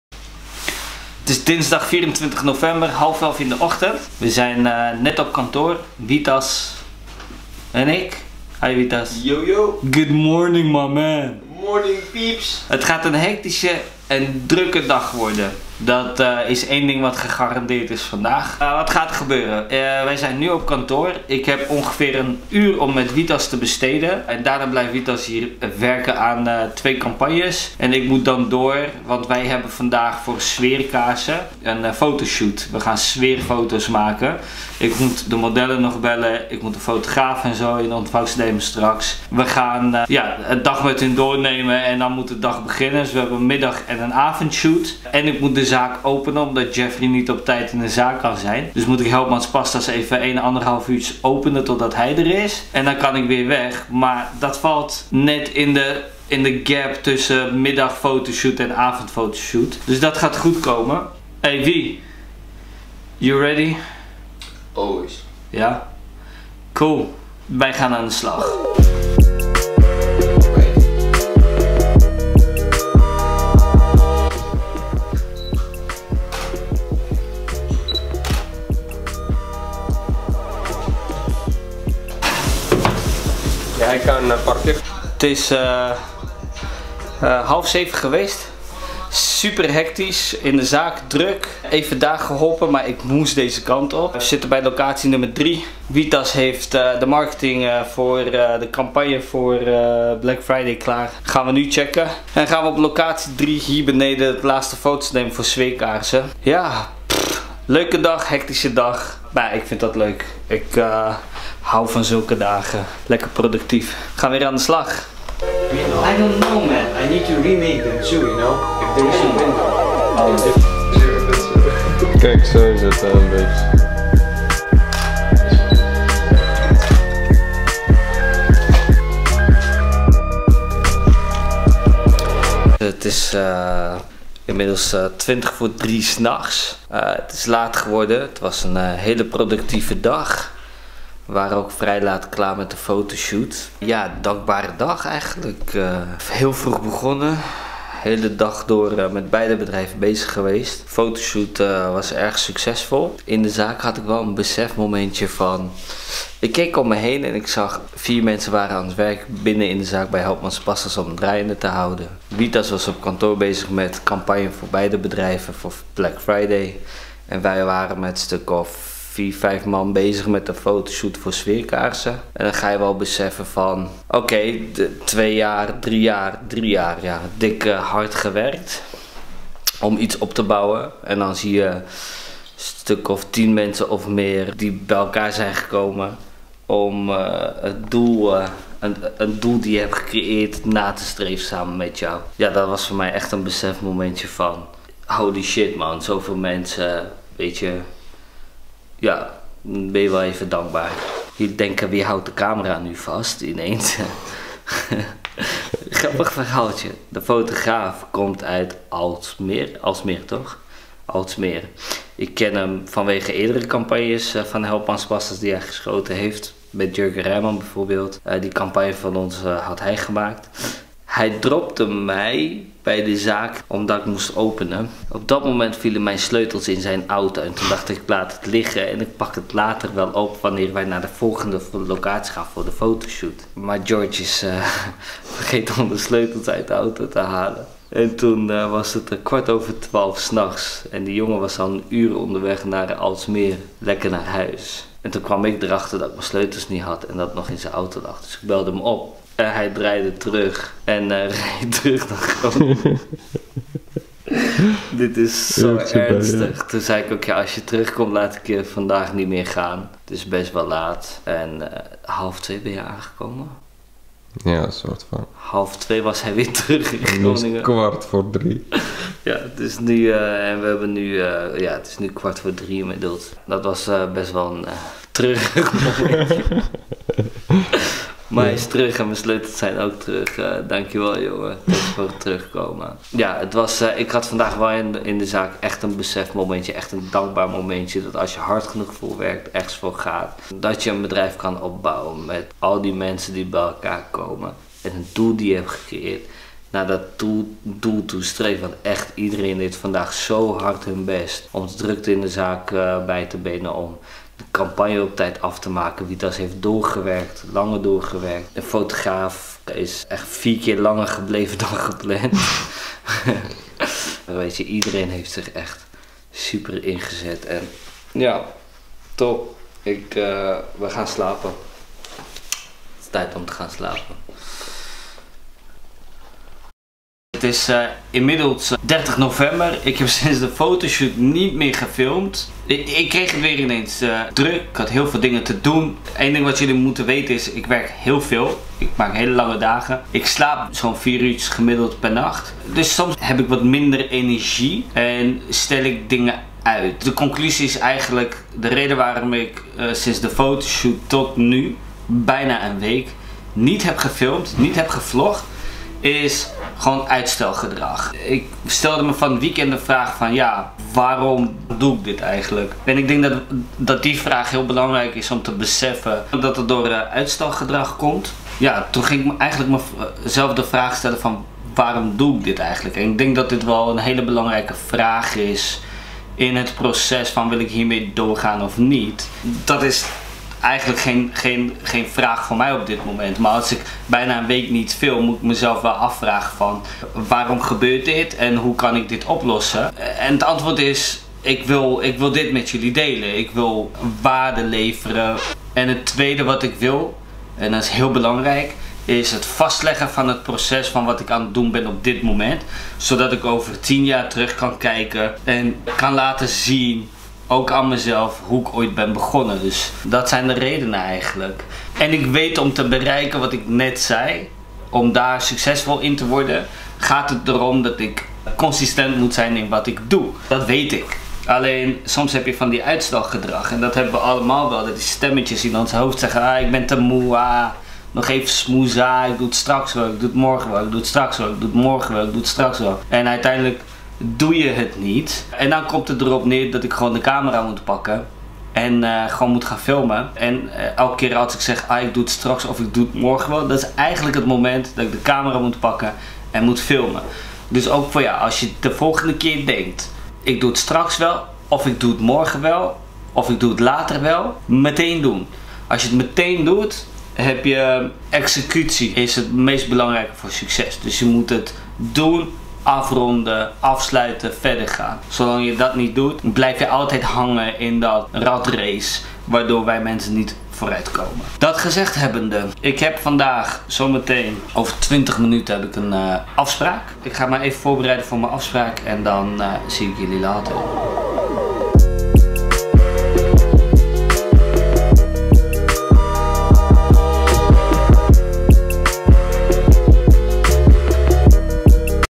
Omgaan met uitstelgedrag | Weekvideo 5
Ik deze weekvideo neem ik jullie mee tijdens de fotoshoot voor Sfeerkaarsen.nl en ontdek ik bij mezelf uitstelgedrag die ik aanpak. Ik vertel je hoe.